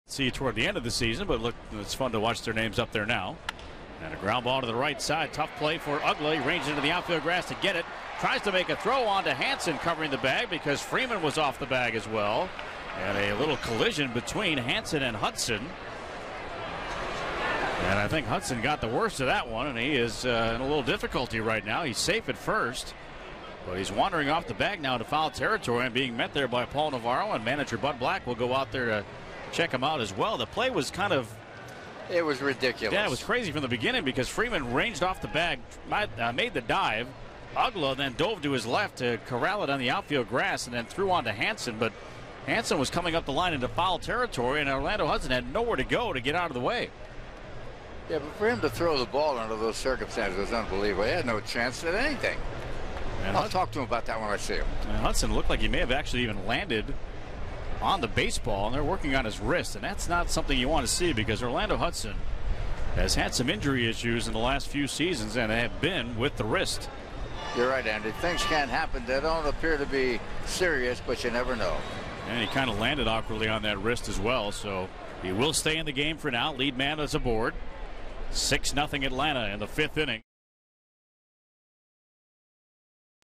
Hudson hurts his wrist
ATL@SD: Hudson collides with Hanson, hurts wrist